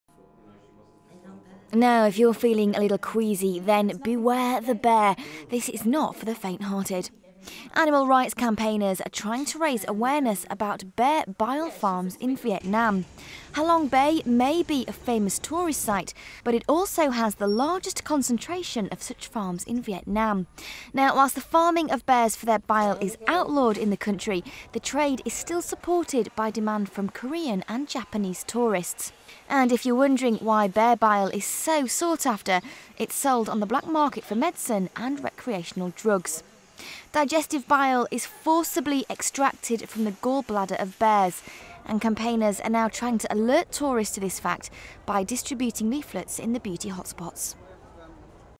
SHOCKING PICTURE: Bear bile used as drug
Campaigners in Vietnam are alerting tourists to the dangers of encouraging the removal of bile from bears for a recreational drug. Report by Louise Hulland.

Will and Kate take their Royal baby home for the first time